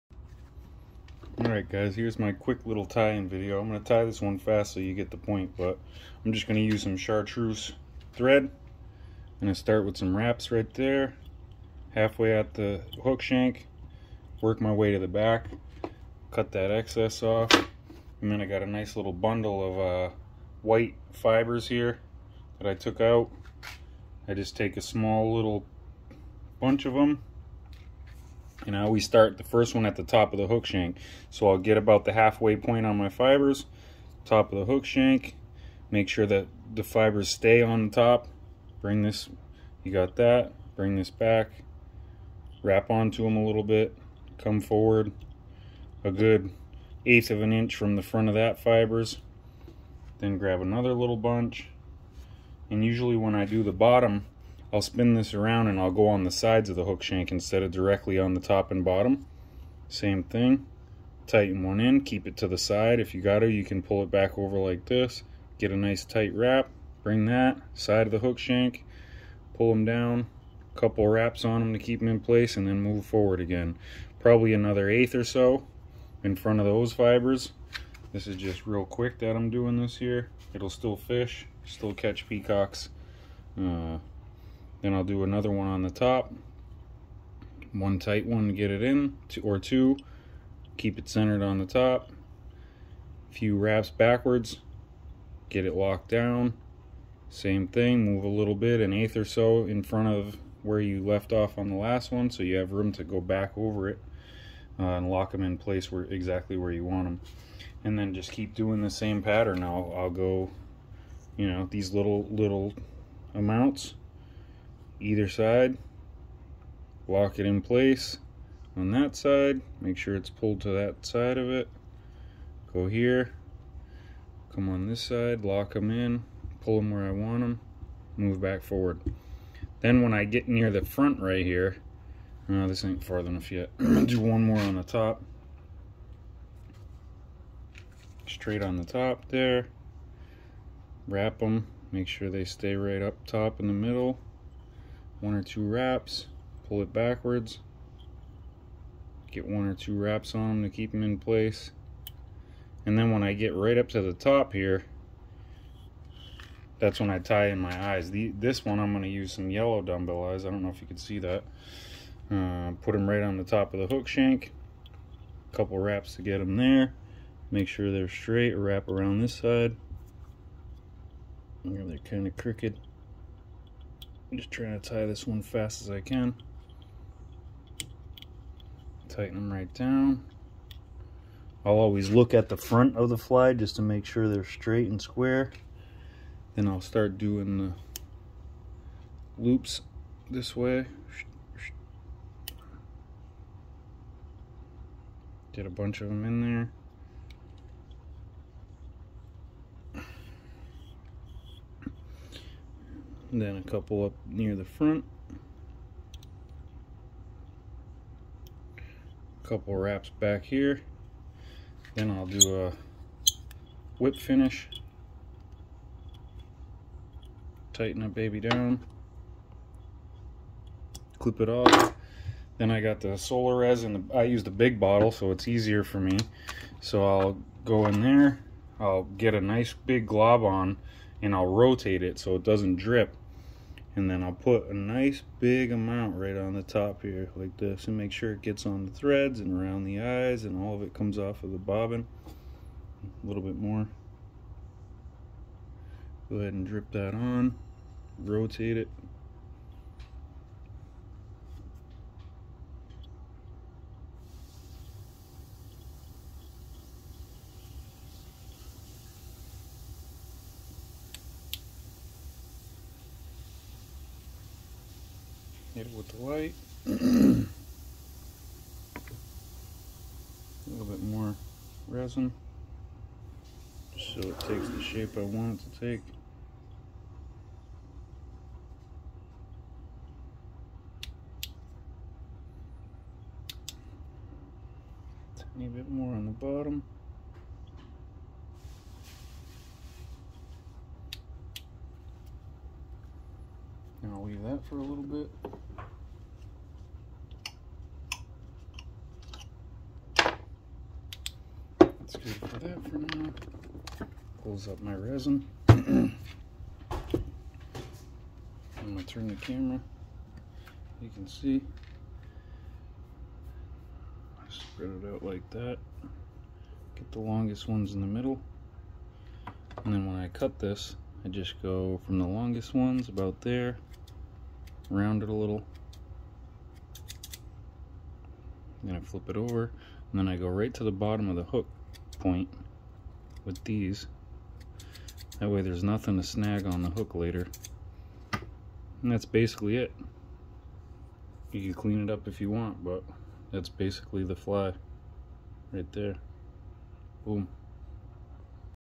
South Florida peacock bass fly tying video
This is just a really simple and very effective fly I use for peacock bass here in south florida.